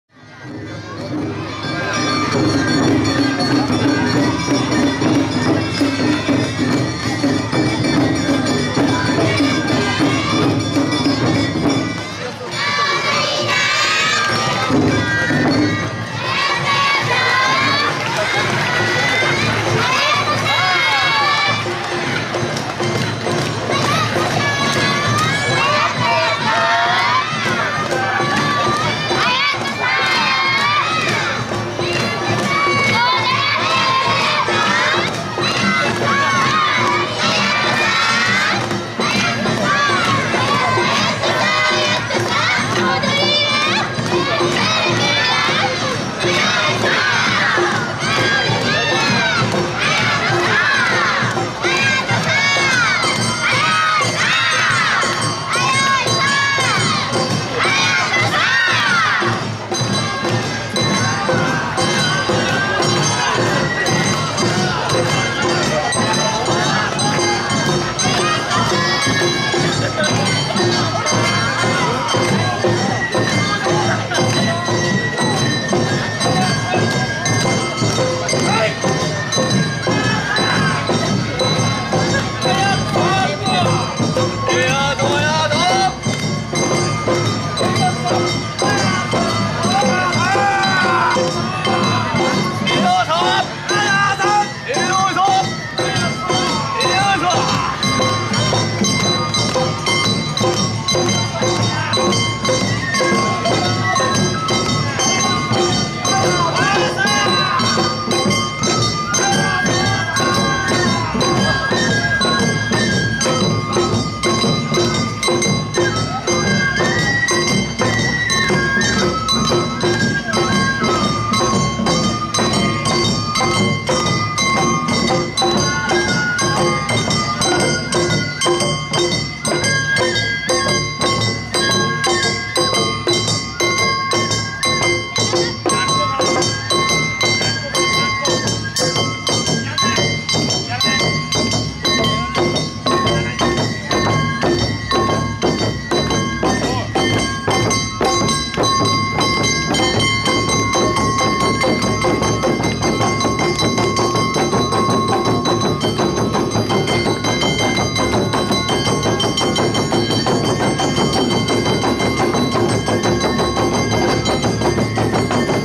「写楽連」夜の流し踊り② 第27回きたまち阿波おどり（2019.7.27）
写楽連の夜の流し踊りです。今年は、初めての第二演舞場（ニュー北町商店街）で撮影。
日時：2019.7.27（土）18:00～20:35
会場：東武東上線  東武練馬駅南口
撮影メモ：FDR-AX700　4K
　　　　　EDIUS Pro 9 で編集